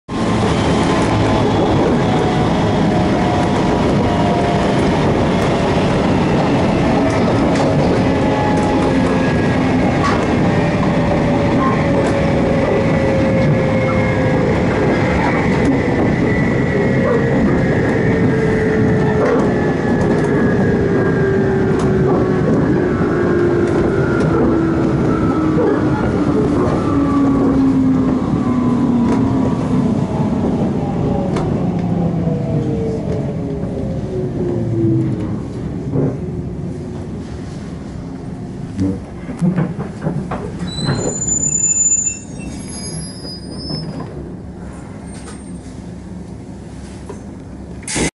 伊達駅に到着する東北本線719系からの車窓
伊達駅に到着する東北本線719系からの車窓です。
東北の冬の山々の景色を見ながら市街地に入り、ホームの長い伊達駅に到着します。となります。